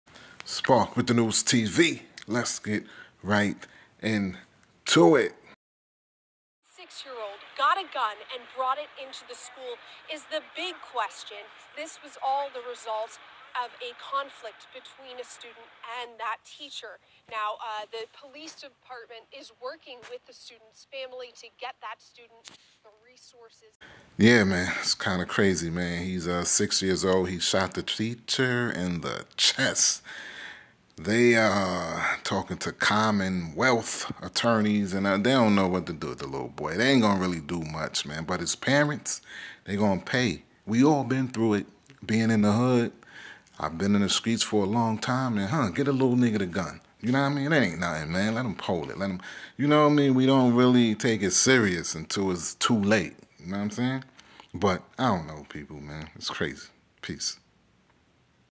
6 Year old shoots teacher in the chest
this video is about the 6 Year old who shot his teacher in the chest